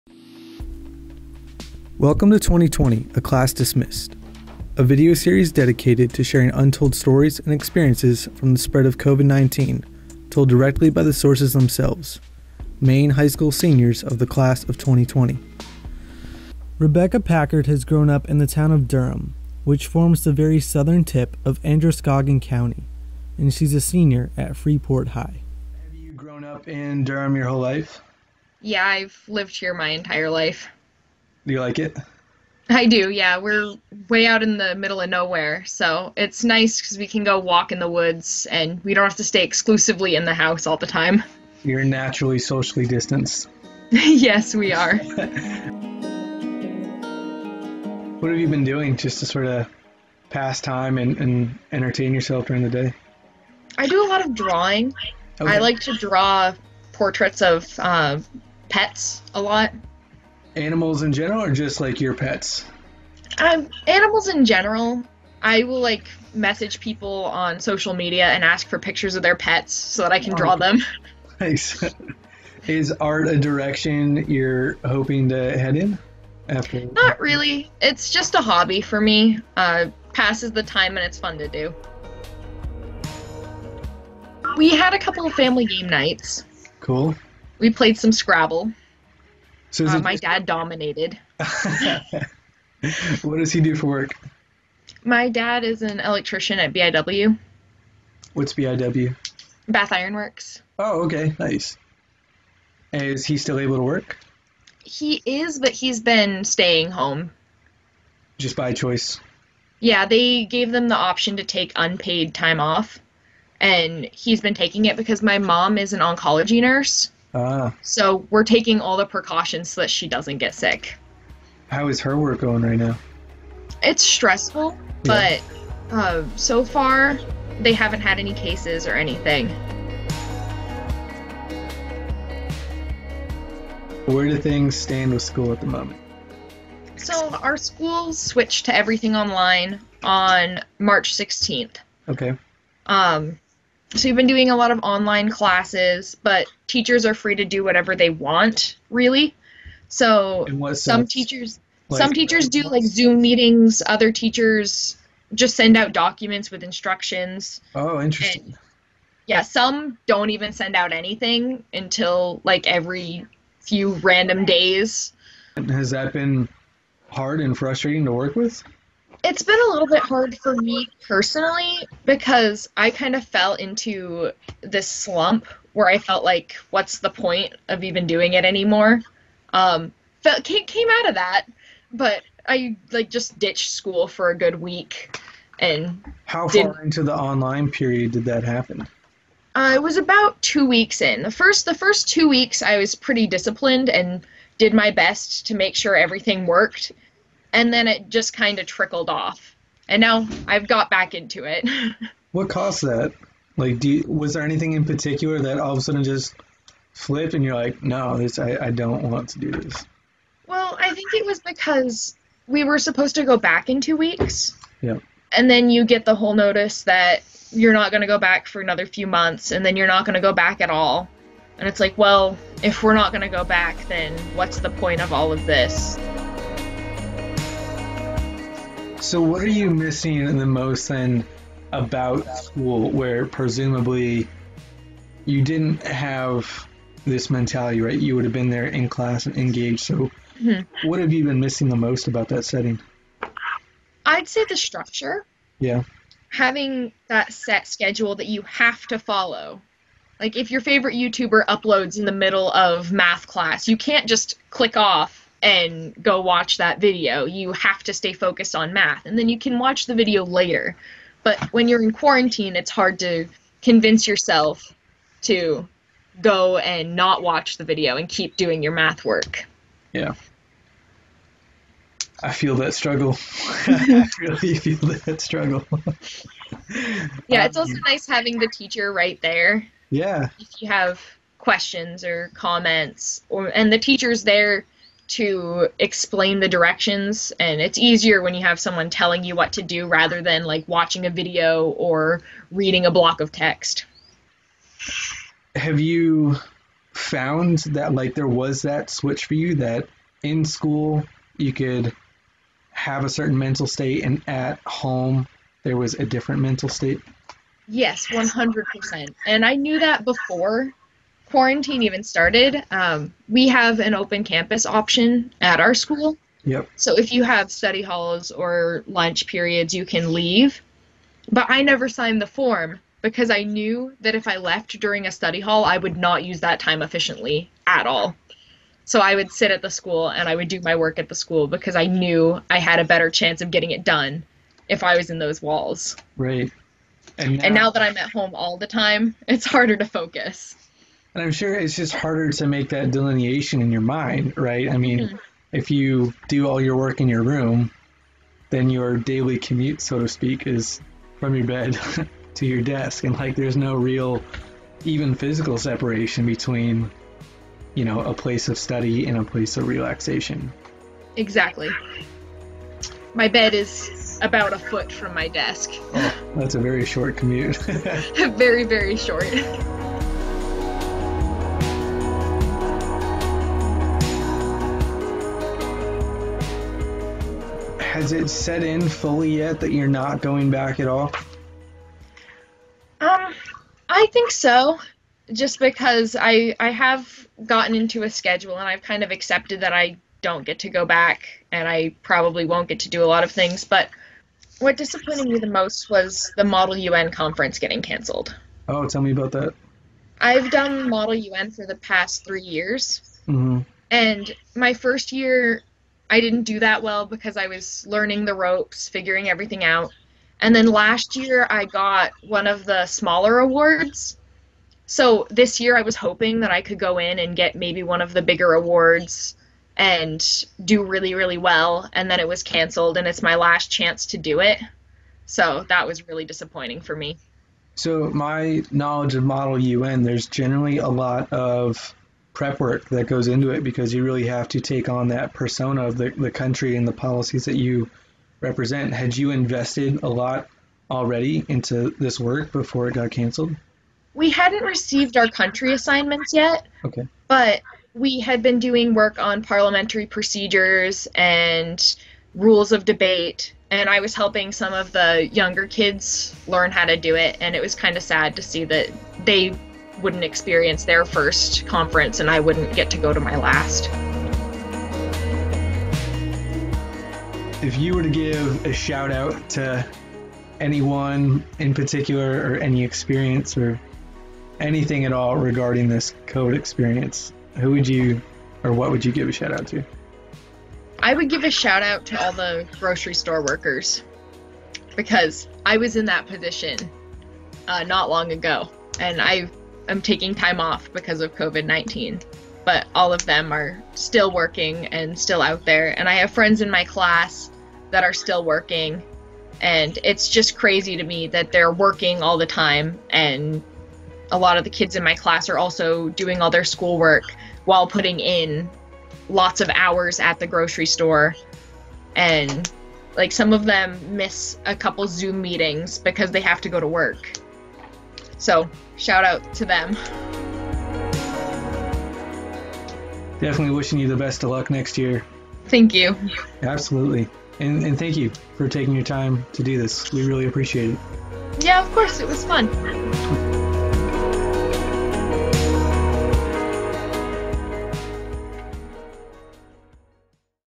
2020: A Class Dismissed - Rebecca Packard, Freeport High School
Rebecca Packard, a senior at Freeport High School, discusses how the coronavirus has impacted her last months of high school — and her work-life balance.

This video, recorded on April 29, is part of Pine Tree Watch's 2020: A Class Dismissed series, documenting the stories and experiences of Maine's soon-to-be-graduates as they deal with the unexpected developments of the COVID-19 pandemic and a disrupted senior year.

Hosted and produced by Bailey Beltramo.